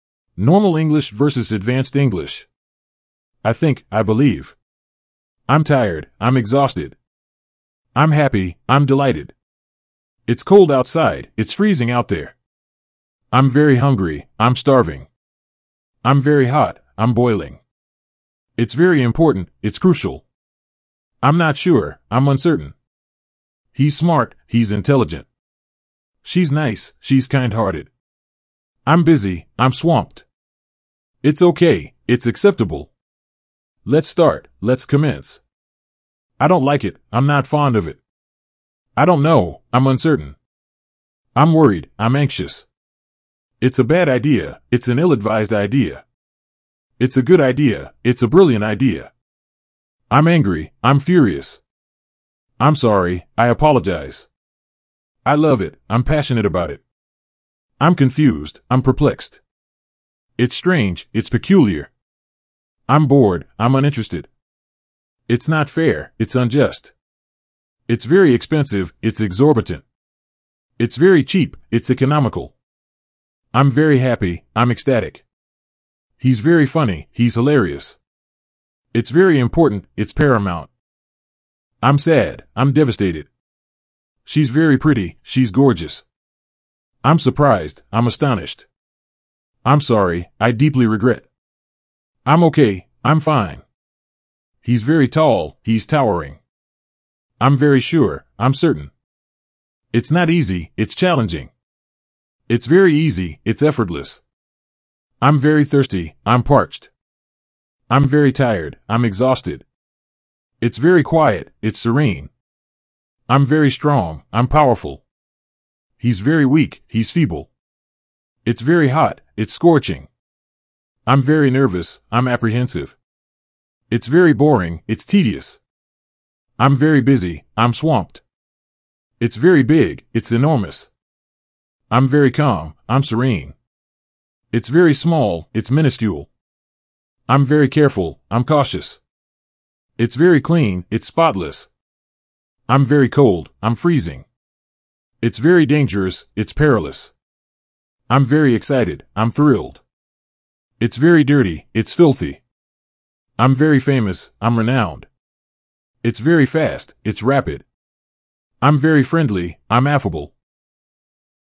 Normal English vs. Advanced English - Part 1
Welcome! In this video, we'll explore how to elevate everyday English into more advanced expressions. From simple phrases to sophisticated alternatives, you'll learn how to sound more professional and refined. Let’s get started!